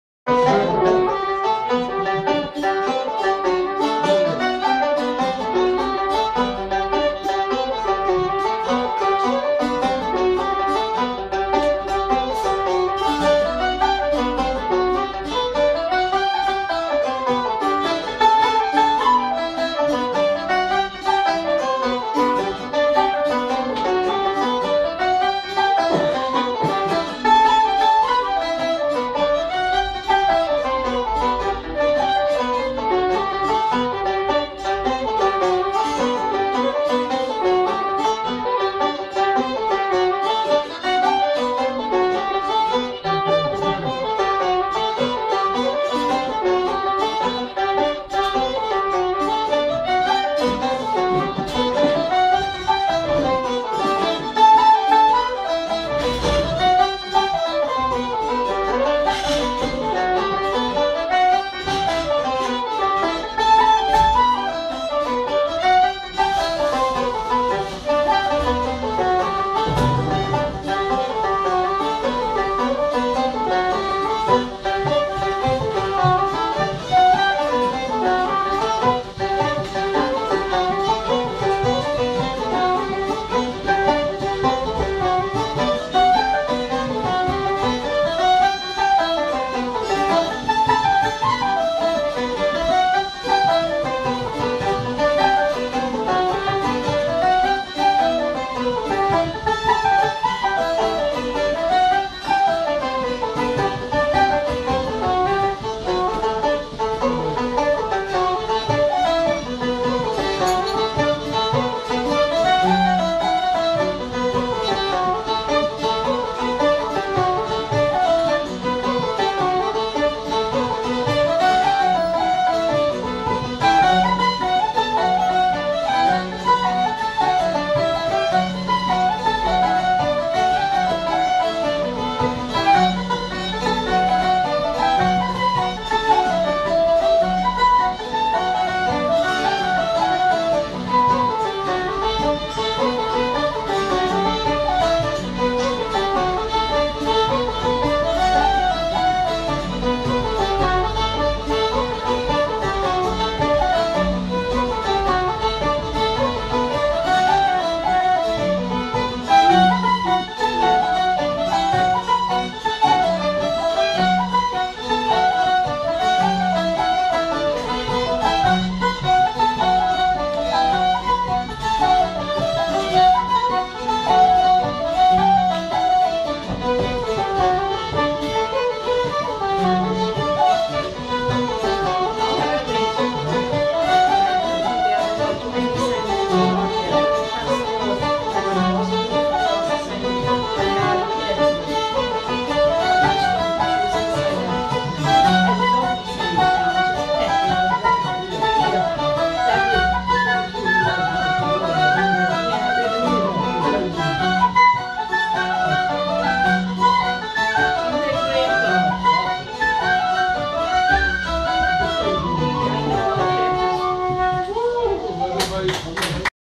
O6- Cruises Sunday Session in Ennis- 10/14/2018
Cruises Sunday Session in Ennis, County Clare on 10/14/2018 lead by Brid O'Gorman. 06